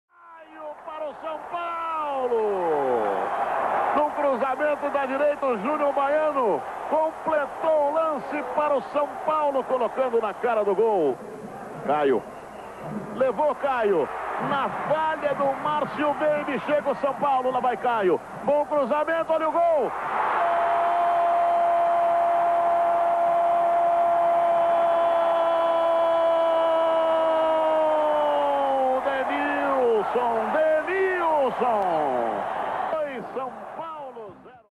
América SP 0 x 2 São Paulo - Narração Luciano do Valle - 1995
Data: Sábado - 25/03/1995
Motivo: 1 fase - Campeonato paulista

Gols:  Caio Ribeiro e Denilson.